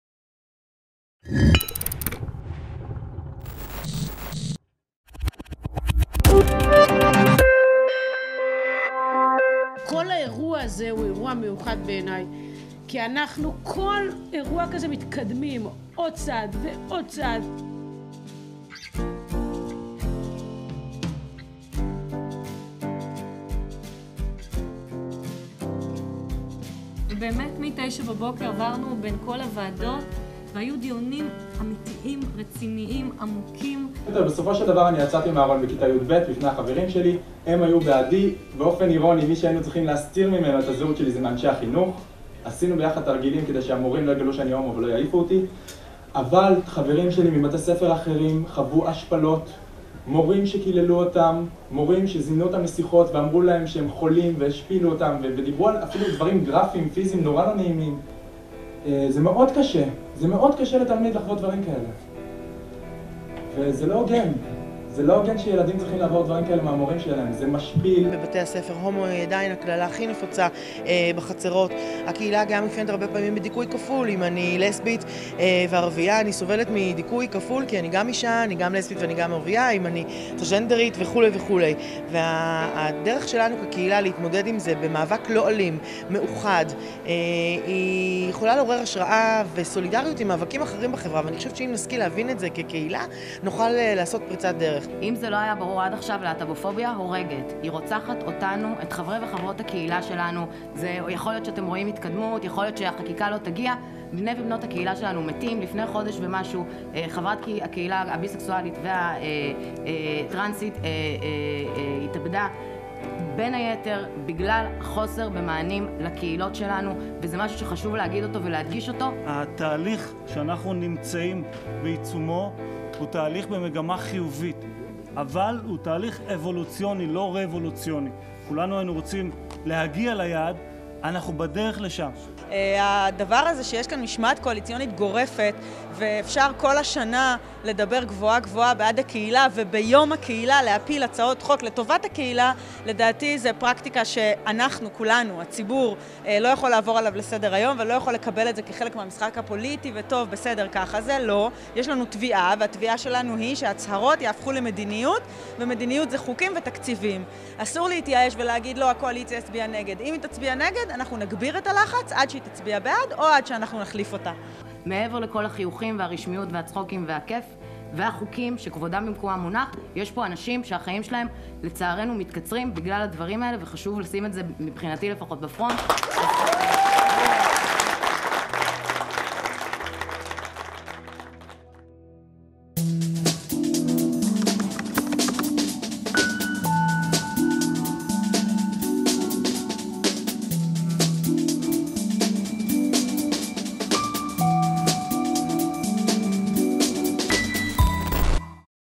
Pride in The Knesset
The Knesset scheduled a day to discuss about the situation of the LGBT community, for the second year. Many committees were held, and Members of Knesset were discussing with LGBT youth associations representatives. However, the same week some laws regarding LGBT community rights weren't approved, thus, is The Knesset really able to provide equality?